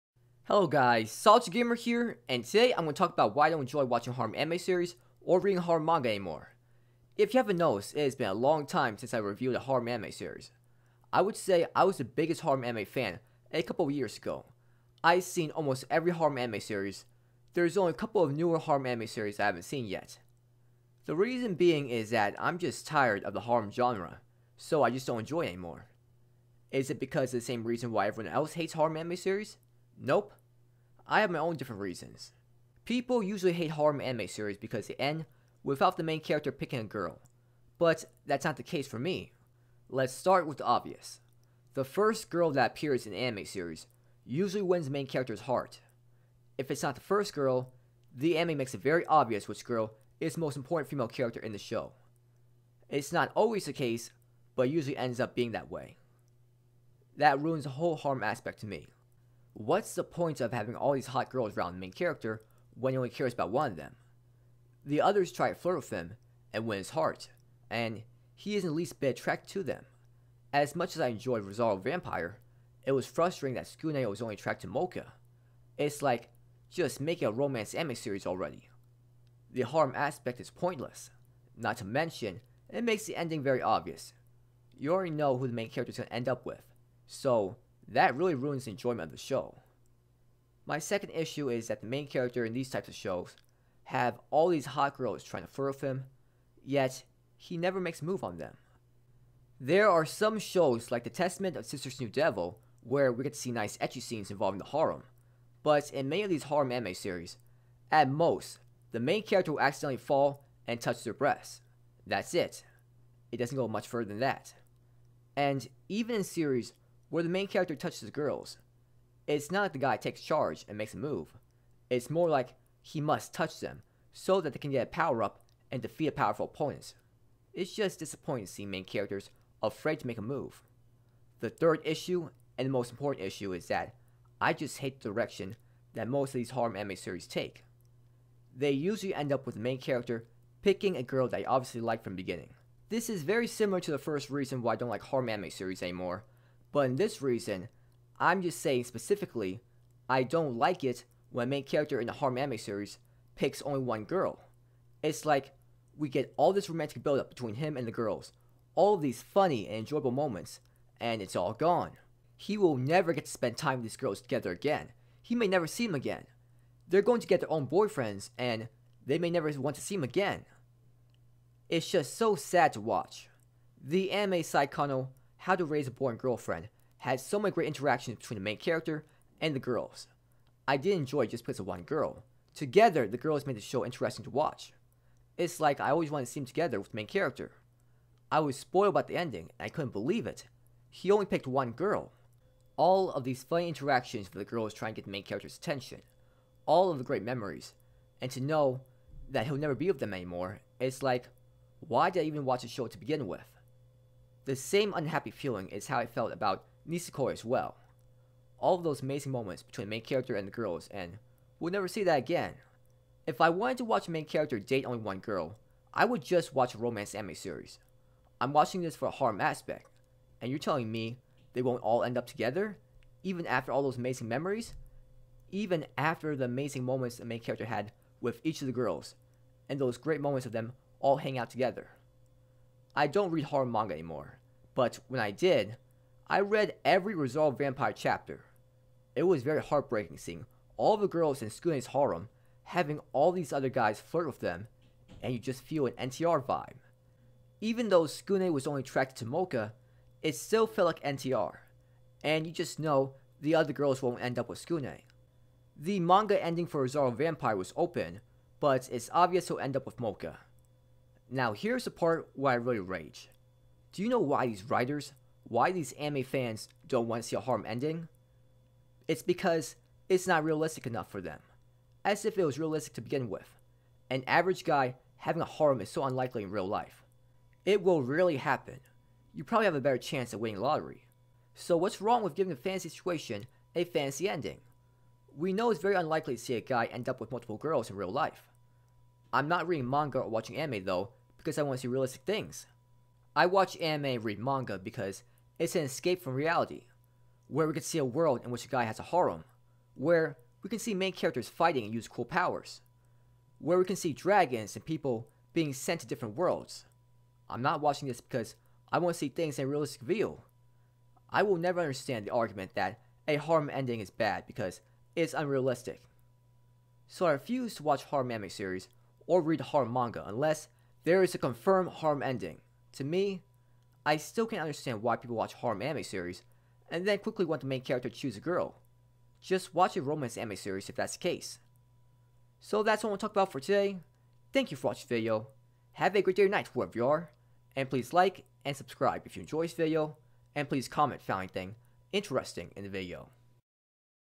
WHY I DON'T LIKE WATCHING HAREM ANIME SERIES ANYMORE!
I Don't Find Harem Anime Series Enjoyable To Watch Anymore